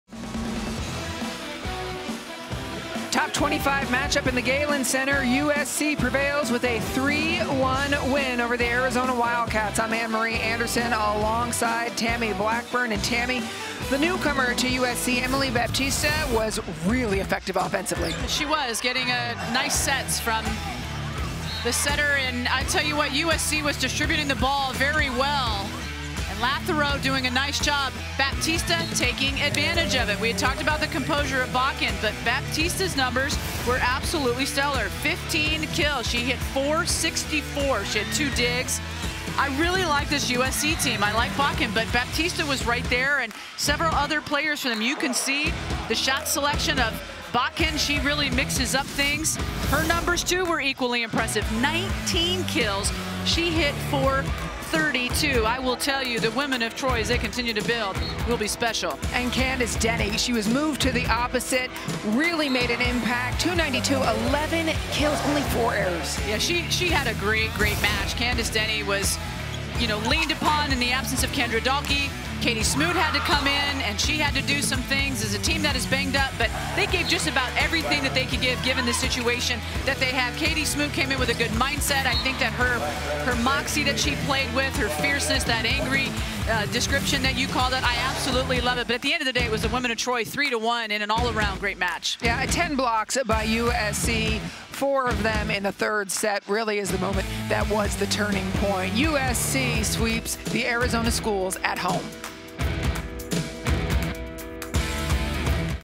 Recap: USC women's volleyball prevails with 3-1 win over Arizona in top-25 matchup
Pac-12 Networks Anne Marie Anderson and Tammy Blackburn recap the top-25 showdown between No. 23 Arizona and No. 15 USC, a matchup that resulted in a Trojan 3-1 win over the Wildcats. USC's Brooke Botkin led the pack with 19 kills, .432 hitting percentage, and had five digs. Arizona's Katie Smoot had 13 kills and hit .219 in Sunday's match.